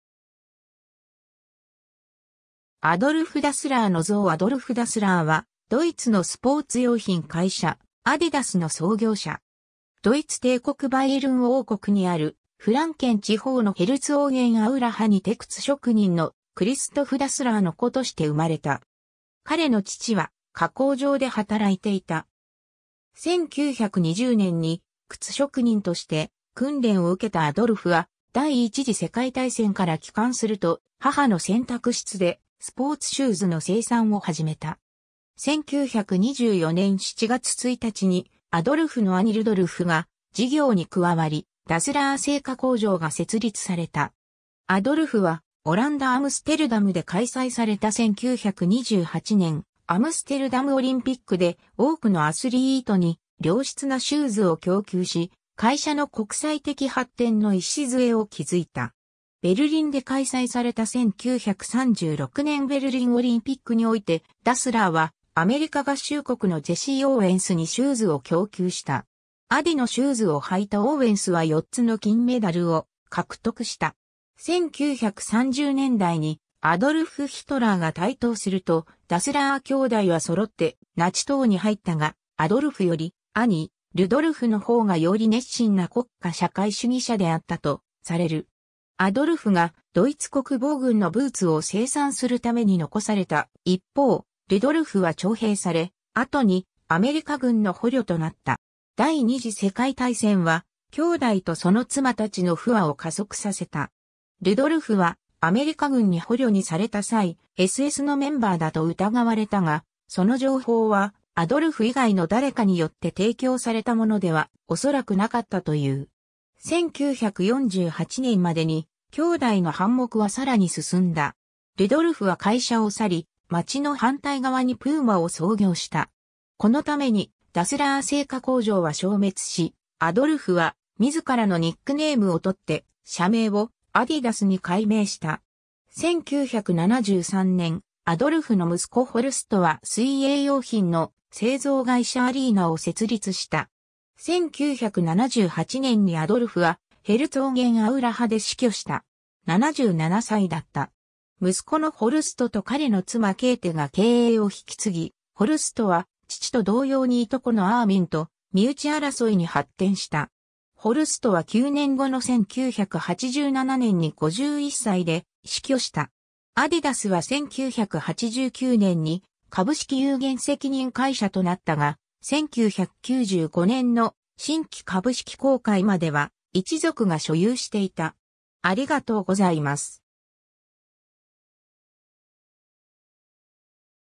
アドルフ・ダスラー
アドルフ・ダスラーの像 アドルフ（アディ）・ダスラー（Adolf "Adi" Dassler、1900年11月3日 - 1978年9月6日）は、ドイツのスポーツ用品会社アディダスの創業者。
 ドイツ帝国バイエルン王国にあるフランケン地方のヘルツォーゲンアウラハにて靴職人のクリストフ・ダスラーの子として生まれた。
彼の父は靴工場で働いていた。
 1920年に、靴職人として訓練を受けたアドルフは、第一次世界大戦から帰還すると、母の洗濯室でスポーツシューズの生産を始めた。
1924年7月1日にアドルフの兄ルドルフが事業に加わり、ダスラー製靴工場(Gebrüder Dassler Schuhfabrik)が設立された。
 アドルフはオランダ・アムステルダムで開催された1928年アムステルダムオリンピックで多くのアスリートに良質なシューズを供給し、会社の国際的発展の礎を築いた。
ベルリンで開催された1936年ベルリンオリンピックにおいて、ダスラーはアメリカ合衆国のジェシー・オーウェンスにシューズを供給した。
アディのシューズを履いたオーウェンスは4つの金メダルを獲得した。
 1930年代にアドルフ・ヒトラーが台頭すると、ダスラー兄弟は揃ってナチ党に入ったが、アドルフより兄ルドルフの方がより熱心な国家社会主義者であったとされる。
 アドルフがドイツ国防軍のブーツを生産するために残された一方、ルドルフは徴兵され、後にアメリカ軍の捕虜となった。
 第二次世界大戦は兄弟とその妻たちの不和を加速させた。
ルドルフはアメリカ軍に捕虜にされた際、SSのメンバーだと疑われたが、その情報はアドルフ以外の誰かによって提供されたものではおそらくなかったという。
 1948年までに兄弟の反目はさらに進んだ。
ルドルフは会社を去り、町の反対側（アウラハ川を挟んだ対岸）にプーマを創業した。
このためにダスラー製靴工場は消滅し、アドルフは自らのニックネーム（アディ・ダスラー）をとって、社名を「アディダス」に改名した。
 1973年、アドルフの息子ホルストは水泳用品の製造会社アリーナ(Arena)を設立した。
1978年にアドルフはヘルツォーゲンアウラハで死去した。
77歳だった。
 息子のホルストと彼の妻ケーテが経営を引き継ぎ、ホルストは父と同様にいとこのアーミン（プーマの2代目社長でルドルフの子）と身内争いに発展した。
ホルストは9年後の1987年に51歳で死去した。
 アディダスは1989年に株式有限責任会社となったが、1995年の新規株式公開(IPO)までは一族が所有していた。